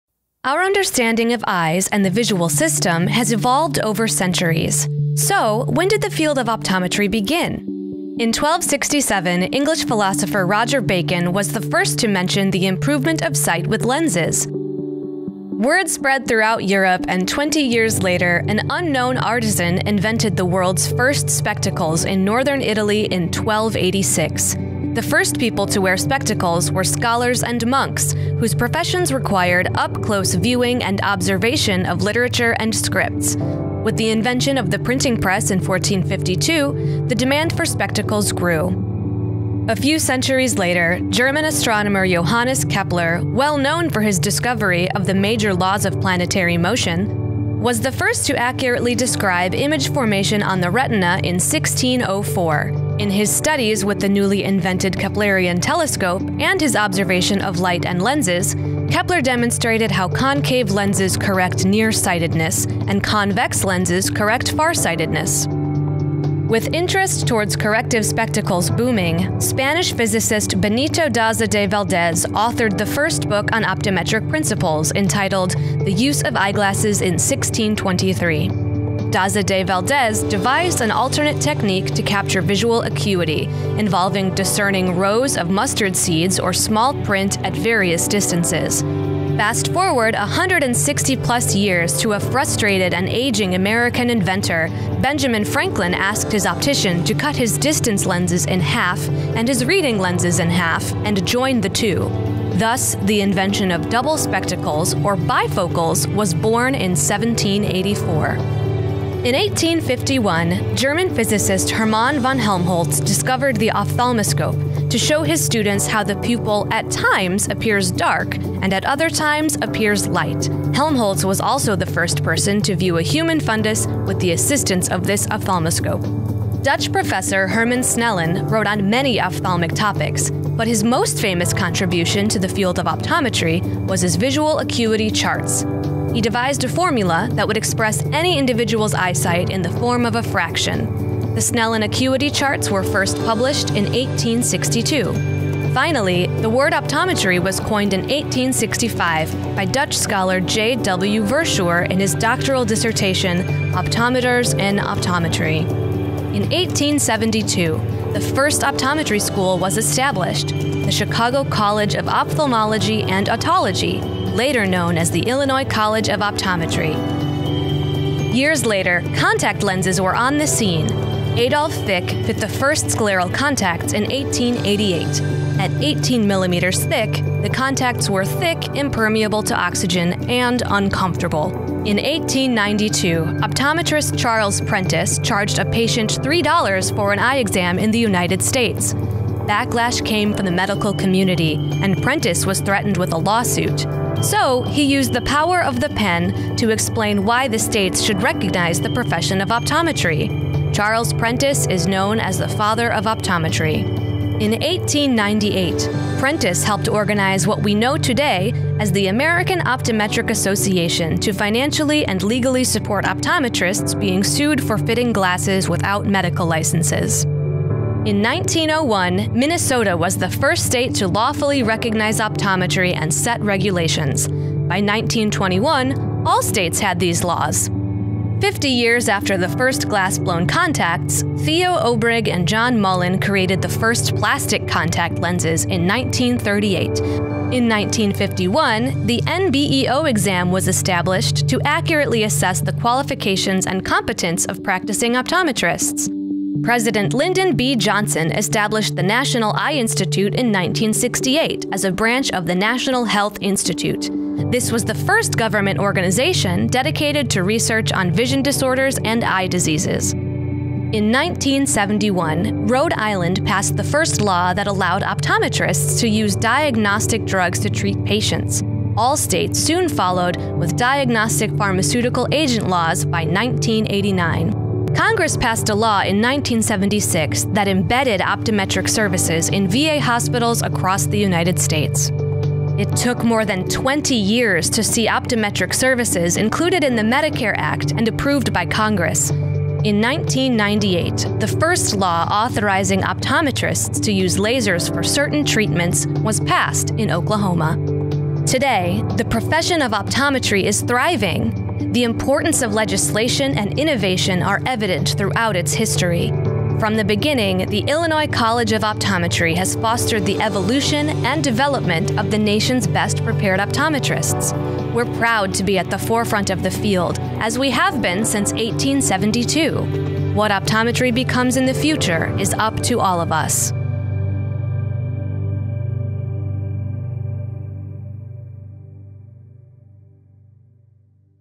ICO presents: The History of Optometry
The profession of optometry may be older than you think! For example, lenses were being used for vision correction as early as the year 1267. This timeline chronicles the history of optometry, from early spectacles to the modern-day Illinois College of Optometry. The future of eye care is clear and bright!